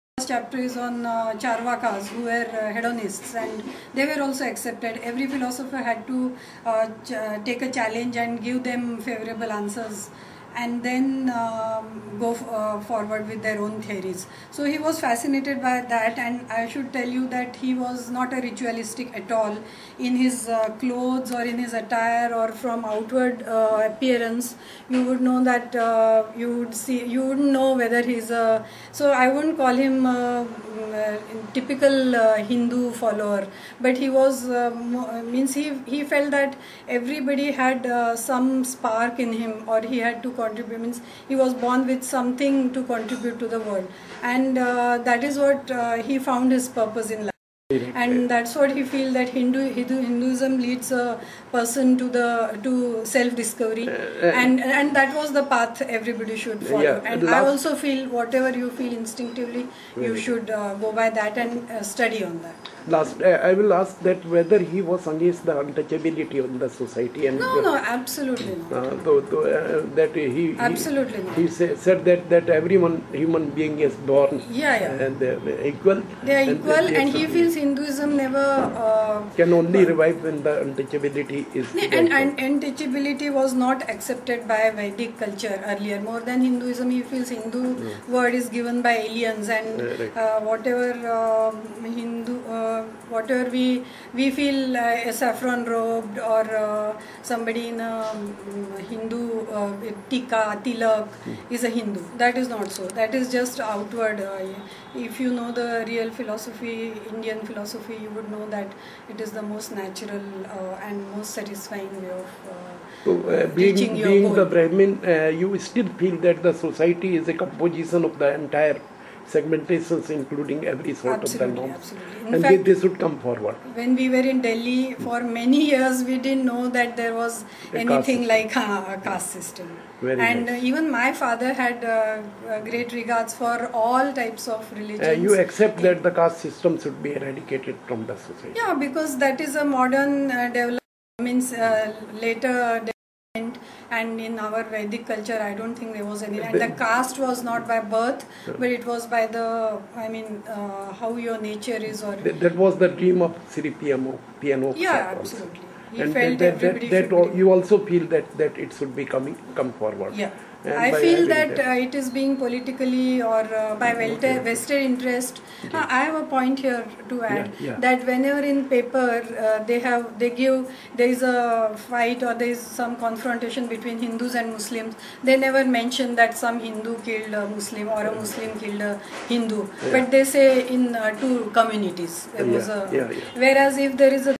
P.N.Oak Was Above Religion and Against Caste Creed.
After reading Shri Oak's work which provides more than adequate evidence to prove that Taj Mahal is indeed Tejo Mahalaya, one has to wonder if the government of Bharat has been full of traitors for the past 50 years! Because to ban such a book which states only the truth is surely a crime against our great nation of Bharat.